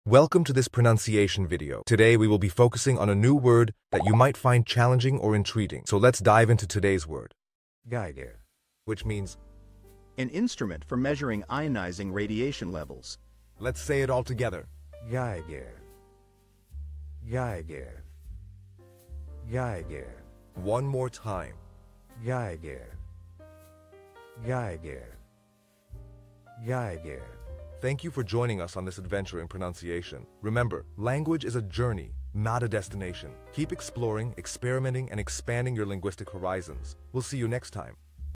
How to pronounce Geiger
Master the Pronunciation of 'Geiger WHICH MEANS GEIGER' - which means : AN INSTRUMENT FOR MASURING IONIZING RADIATION LEVES. 📝 with @PortuguesePronunciation 🎓🗣️ - [Your Guide to Portuguese]

Learn to pronounce 'Geiger' perfectly in Portuguese with our easy and engaging tutorial by @PortuguesePronunciation! 📖📽️ In this comprehensive video, we provide you with native speaker examples 🗣️👂 and phonetic breakdowns 🎵 to ensure you get the pronunciation just right.

How to say Geiger in Portuguese